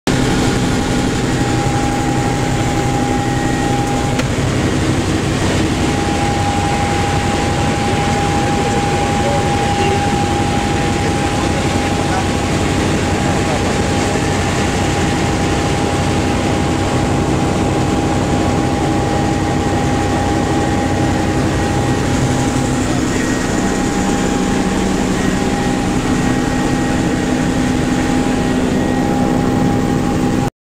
Flying Bell 230 over Canaima Venezuela
Pepito, Freddy Garcia Lujan, Oscar and William Marquina